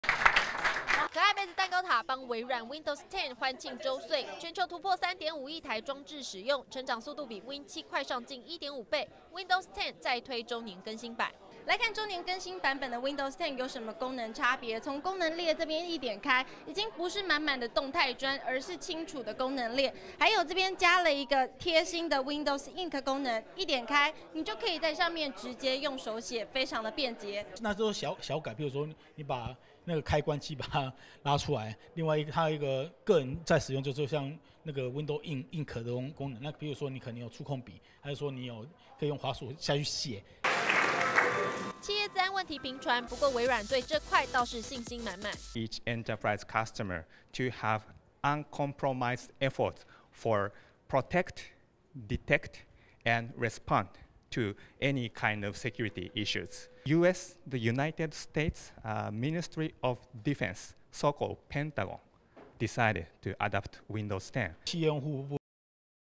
Win10一歲了! 微軟慶生推免費更新
微軟推win10現在又出新招！微軟宣布今天開始，Win10用戶可透過微軟更新或Win 10 ISO檔 進行免費周年更新，大動作分享優惠，就是要刺激用戶升級意願!